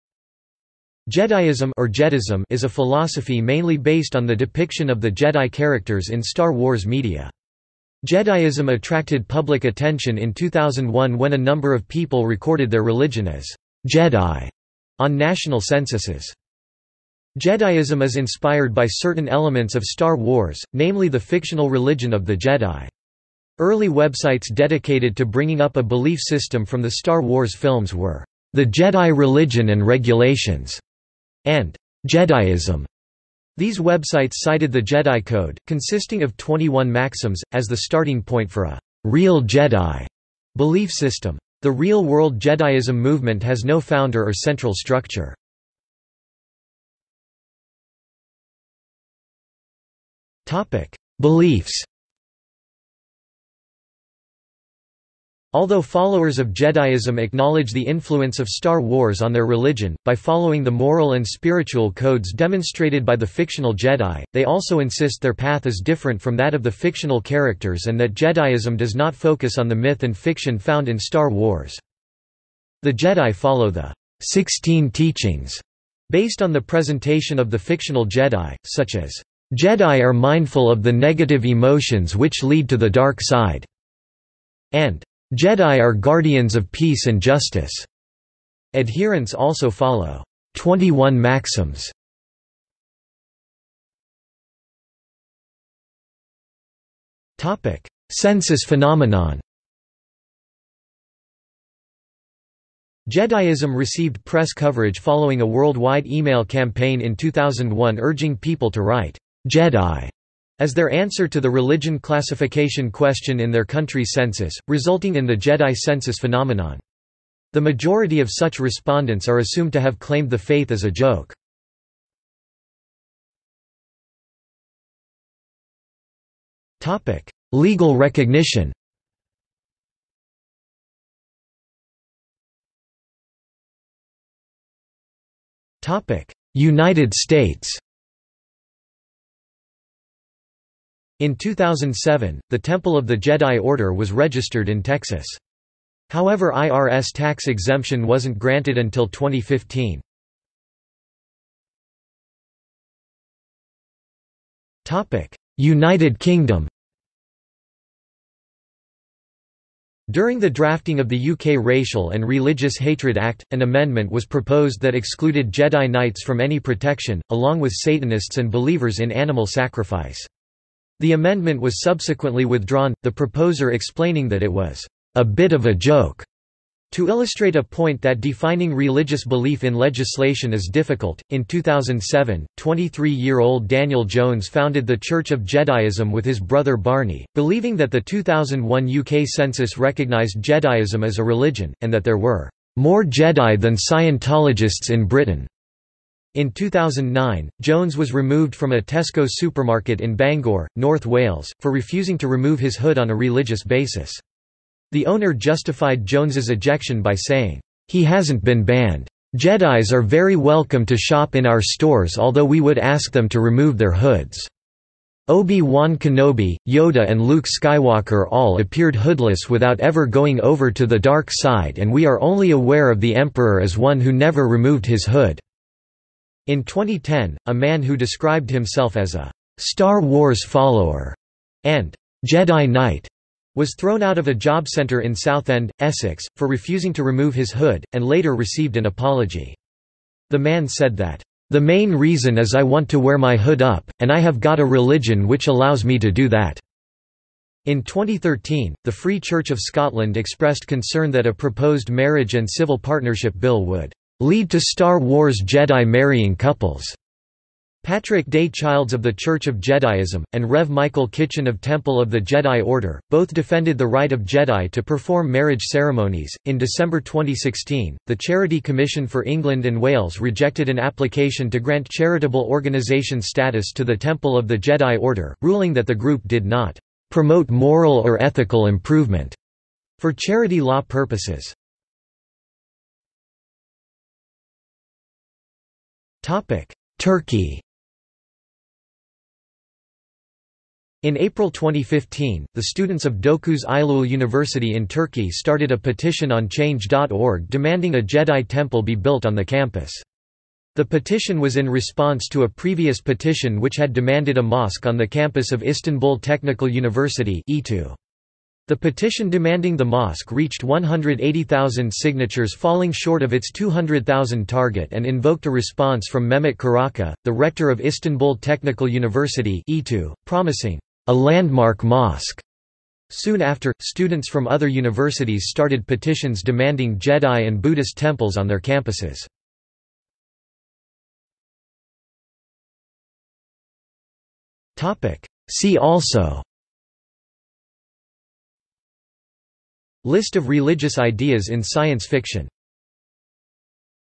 Jediism | Wikipedia audio article
This is an audio version of the Wikipedia Article:
Jediism

00:00:46 1 Beliefs
00:01:33 2 Census phenomenon
00:02:03 3 Legal recognition
00:02:12 3.1 United States
00:02:31 3.2 United Kingdom
00:04:42 3.3 Turkey
00:05:31 4 See also

- Socrates

SUMMARY
Jediism (or Jedism) is a philosophy mainly based on the depiction of the Jedi characters in Star Wars media. Jediism attracted public attention in 2001 when a number of people recorded their religion as "Jedi" on national censuses.
Jediism is inspired by certain elements of Star Wars, namely the fictional religion of the Jedi. Early websites dedicated to bringing up a belief system from the Star Wars films were "The Jedi Religion and regulations" and "Jediism". These websites cited the Jedi code, consisting of 21 maxims, as the starting point for a "real Jedi" belief system. The real-world Jediism movement has no founder or central structure.